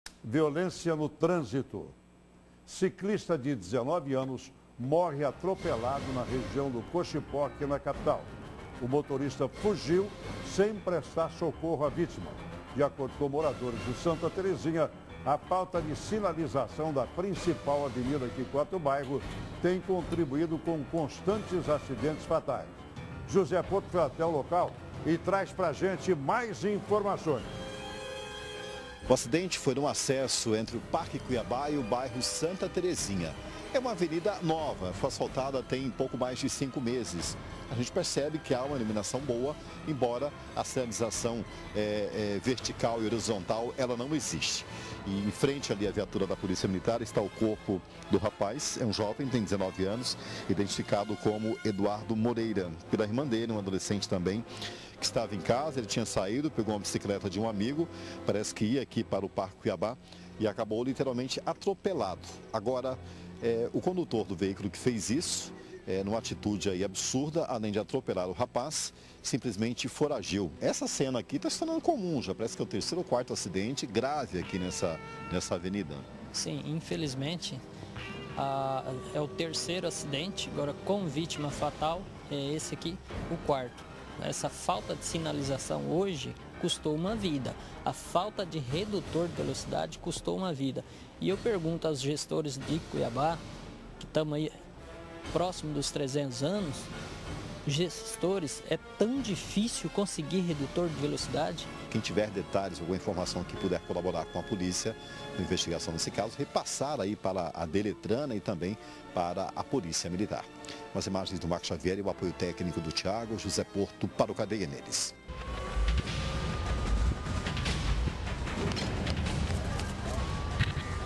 Homem atropela e mata ciclista e foge
Fato aconteceu na região do Coxipó, em Cuiabá. A vítima tinha apenas dezenove anos.